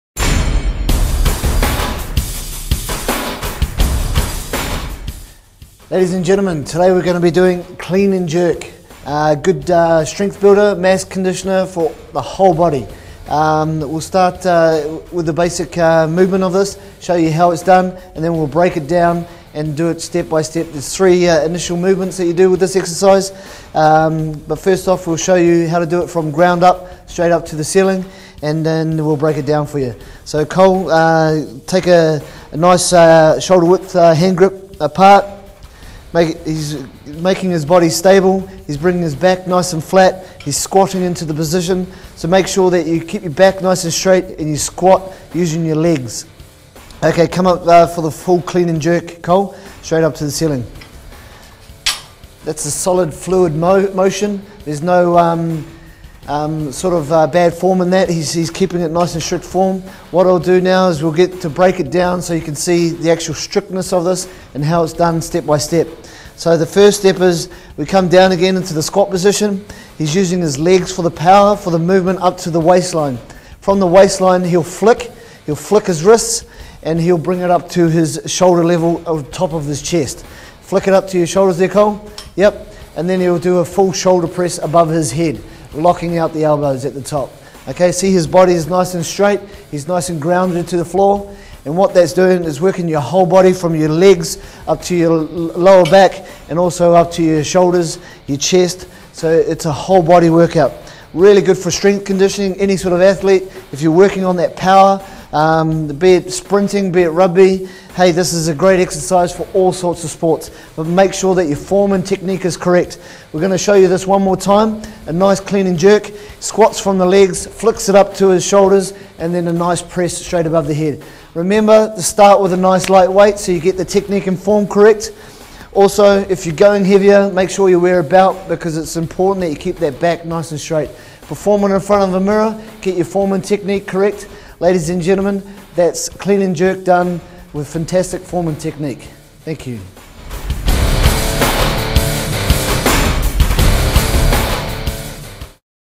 Clean And Jerk
- Powerlifting Exercises & Training - Clean & Jerk - Great for strength and conditioning, the Clean & Jerk is a dynamic exercise which trains the entire body. Presented by Mr New Zealand Alex Simays & Colm Wolfe - NZ's Strongest Man